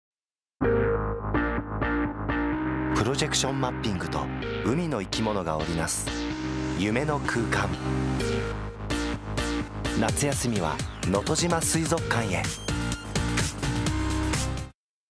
【のとじま水族館】のと海遊回廊_夏休みCM
石川県の七尾湾に浮かぶ能登島。島を代表する観光スポットのひとつが、のとじま水族館だ。同館では、能登半島近海に生息、または回遊してくる魚を中心に展示。日本海側で唯一、ジンベエザメに会える水族館としても人気の施設を動画で紹介！

▼のとじま水族館 紹介記事【ウォーカープラス】